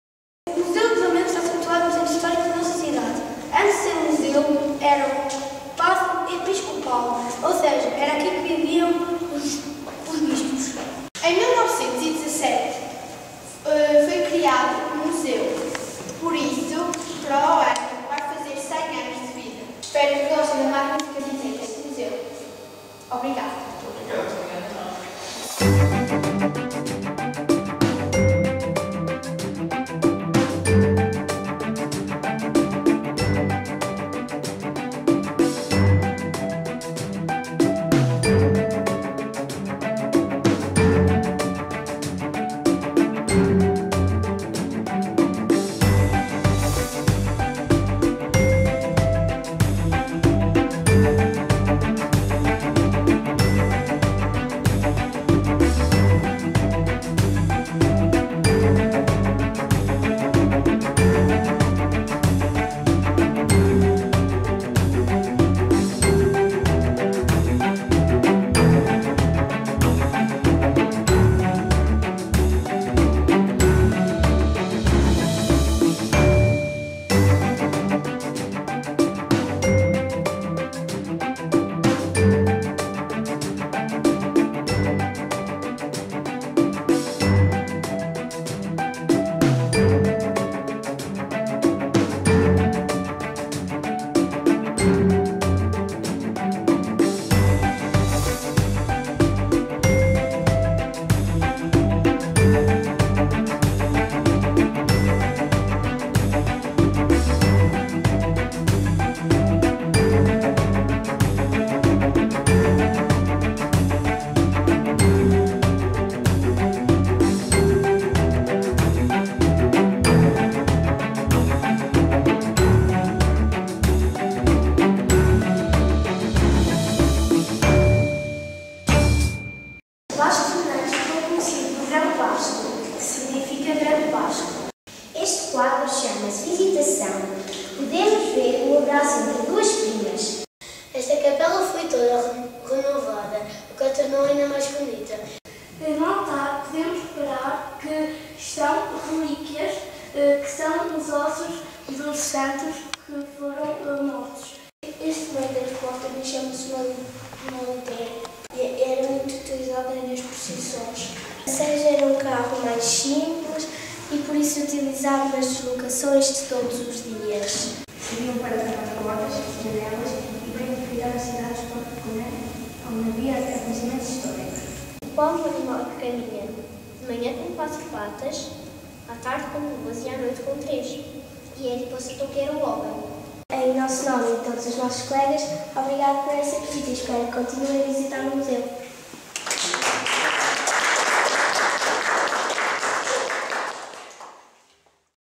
Guias de Palmo e Meio no Museu de Lamego
No passado dia 4 de junho de 2016 os colaboradores do Museu de Lamego foram crianças e a eles coube a responsabilidade de zelar pelo bom funcionamento do museu. A iniciativa inseriu-se nas comemorações do Dia da Criança e resultou de uma parceria entre o Museu de Lamego, Liga dos Amigos do Museu de Lamego e Agrupamento de Escola da Sé-Lamego.

Este é um projeto que procura sensibilizar a comunidade para a importância da preservação e valorização do património, ao mesmo tempo que pretende despertar as crianças para o importante e indispensável compromisso que é cuidar de um espaço de arte, cultura e memória.